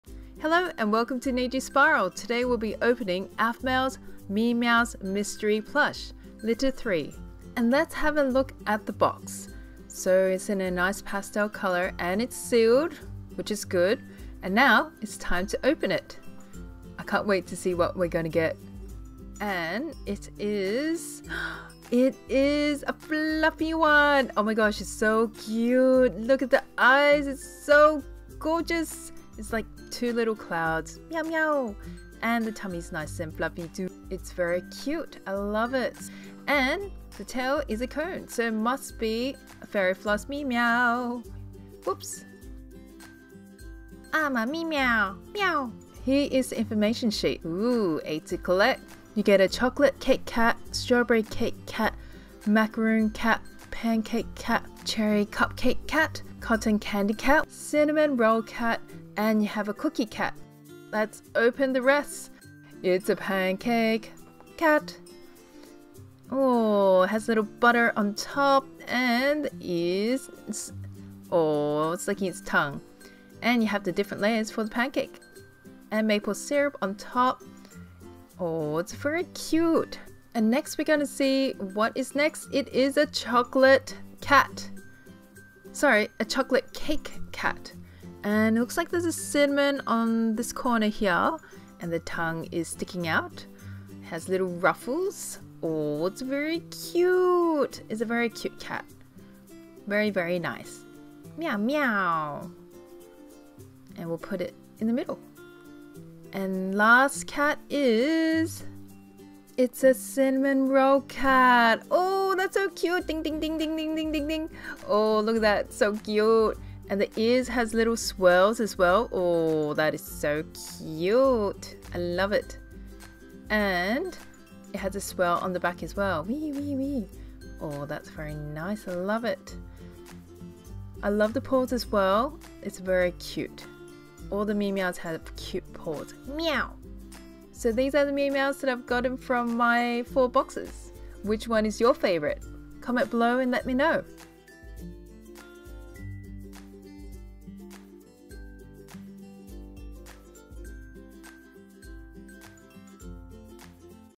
Unboxing Aphmau MeeMeows Litter 3
Unboxing 4x Aphmau MeeMeows Litter 3. It has a cute sweet food/dessert theme.

Time Stamps:
00:00 - Intro
0:08 - 1st Cat
0:47 - Info Sheet
1:01 - 2nd Cat
1:21 - 3rd Cat
1:47 - 4th Cat